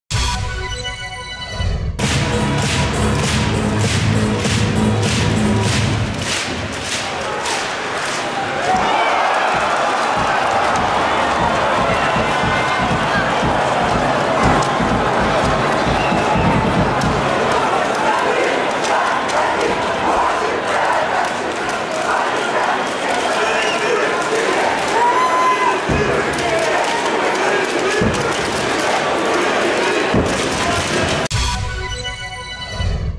Эрик Густафссон в Новосибирске забивает сотый гол "Авангарда" в сезоне!
Шайба Эрика Густафссона "Сибири" в первом периоде стала сотой для нашей команды в текущем регулярном чемпионате КХЛ.